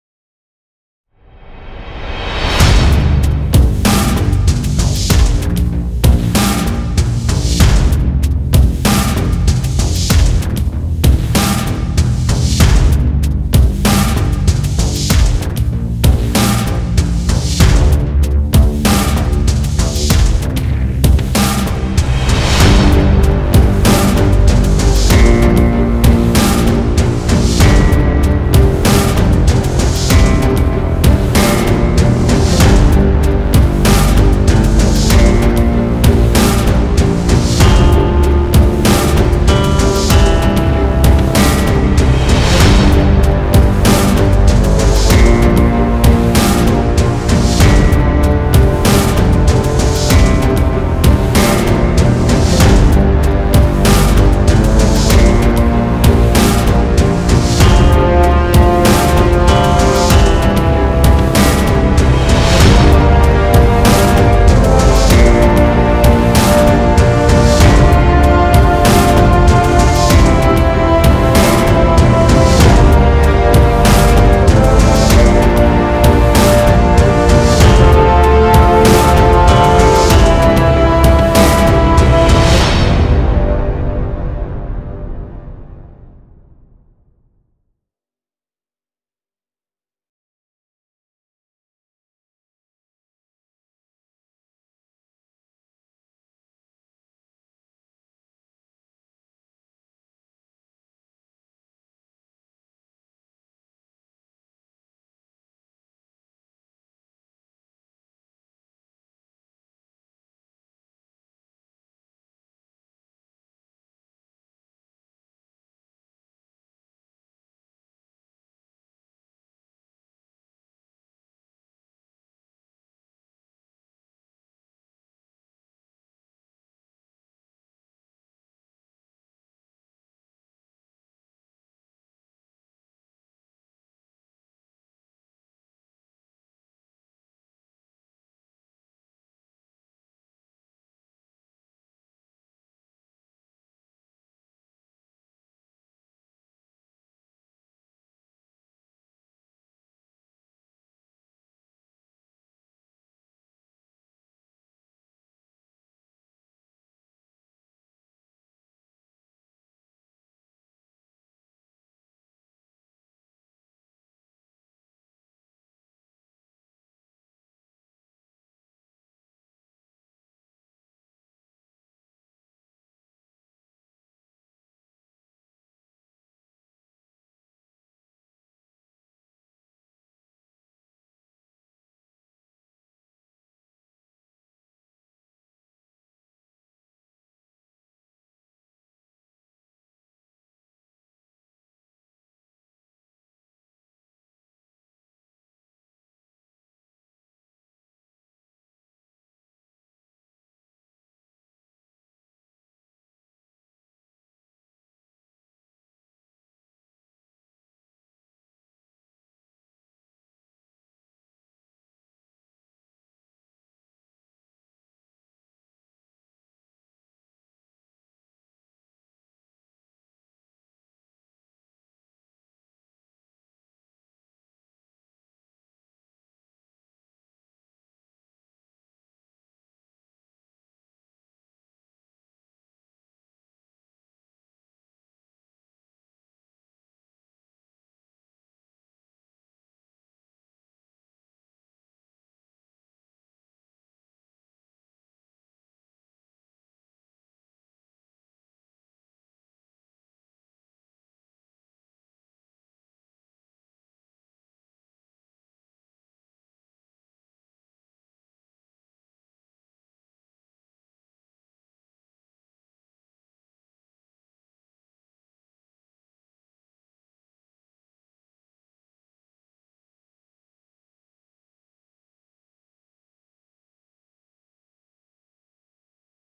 Parrot BEBOP 2 Drone Flight test
Segundo vuelo con el nuevo drone de parrot; el BEBOP 2. La calidad de la imagen es bastante mala, pero las cualidades de vuelo son alucinantes.